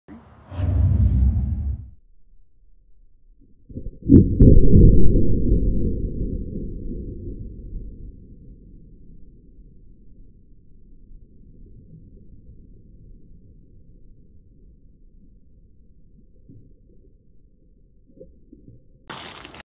Shooting a water balloon with a 25 cal air rifle
As stated in the title. Hatsan 125 25 cal